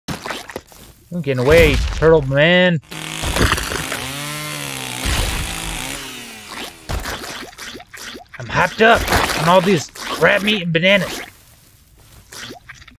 I'm hyped up on crab meat and bananas (king of crabs)
All this crab meat and bananas is making me feel oh so powerful. Won't you join me on my quest to conquer all?